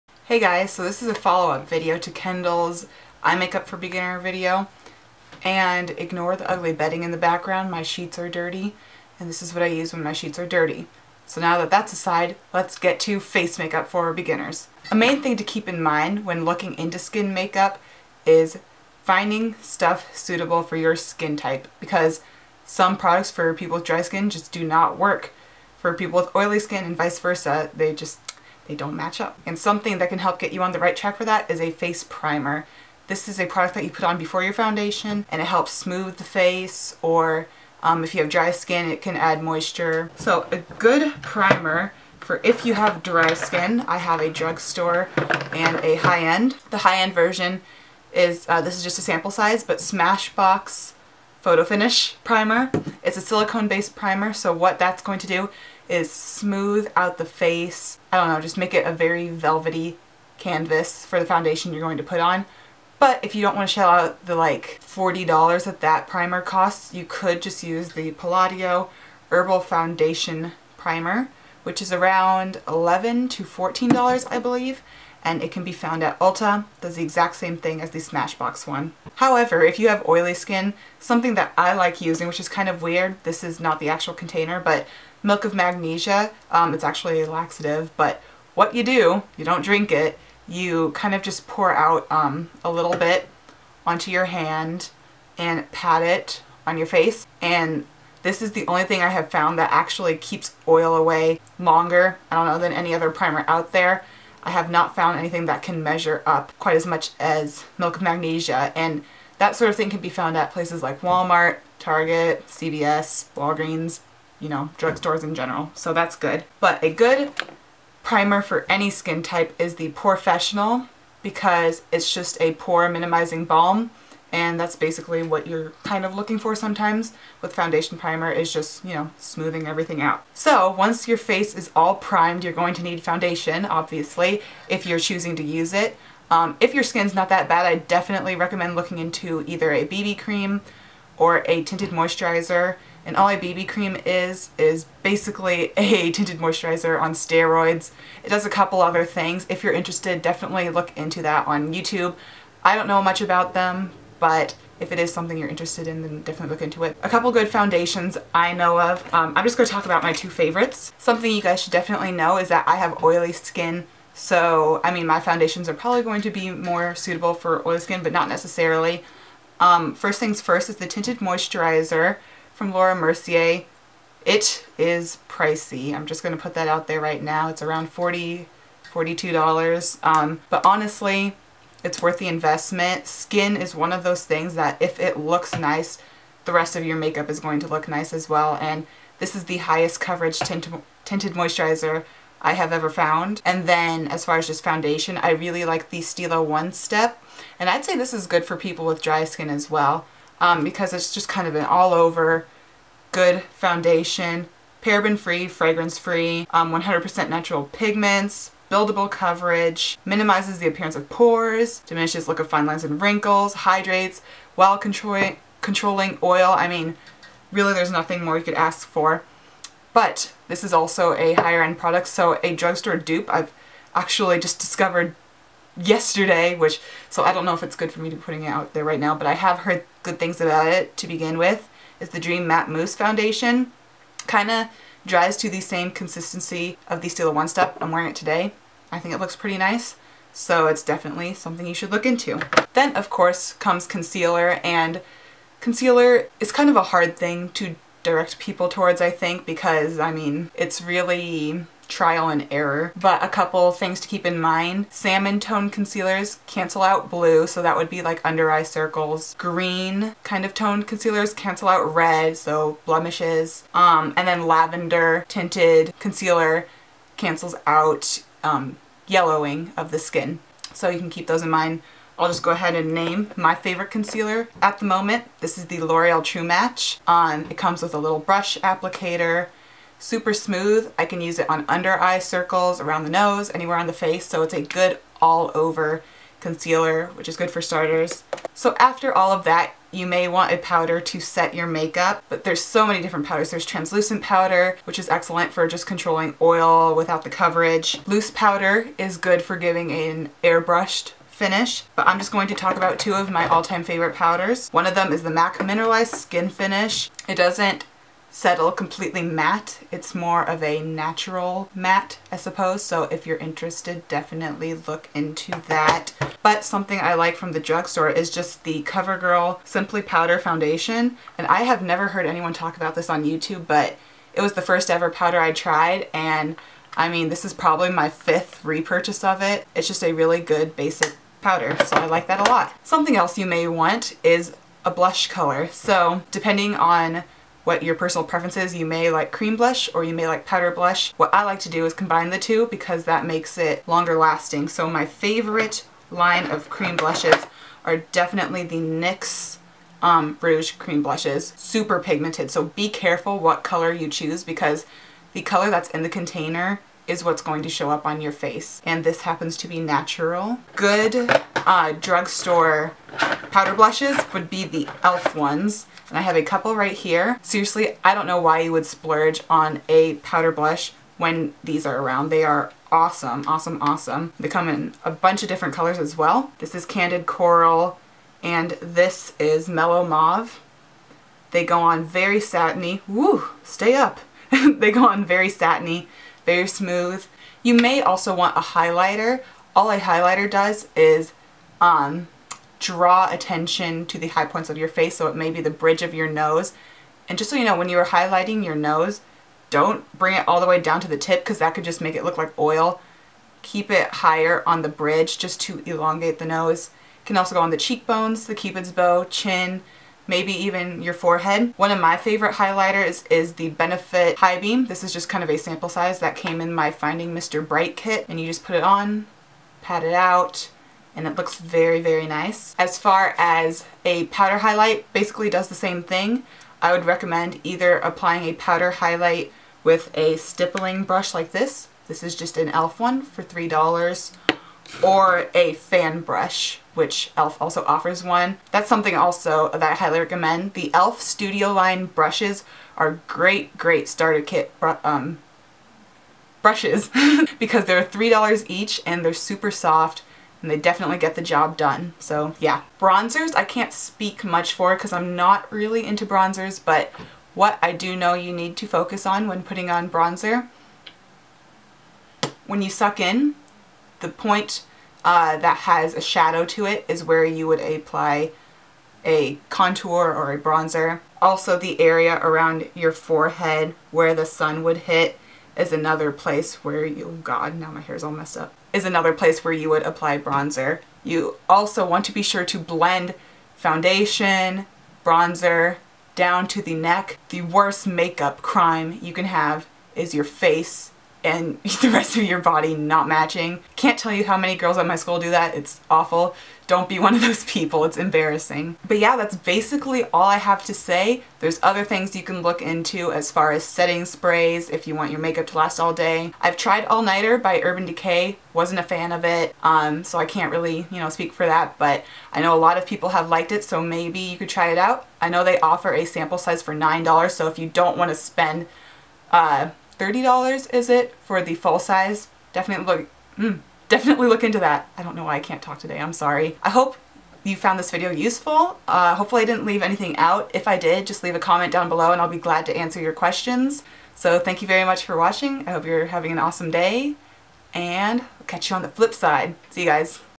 Face Makeup for Beginners
[][][][][]Product List[][][][][]

Primers-
Smashbox Photo Finish
Palladio Herbal Foundation Primer
Milk of Magnesia
Benefit's The Porefessional

Foundations-
Laura Mercier Tinted Moisturizer (oil free)
Stila One Step Makeup
Maybelline Dream Matte Mousse

Concealer-
L'Oreal True Match Super-Blendable Concealer

Powders-
MAC Mineralize Skinfinish Natural
Covergirl Simply Powder

Blushes-
NYX Rouge Cream Blush
E.L.F. Studio Blush

Setting Spray-
Urban Decay All Nighter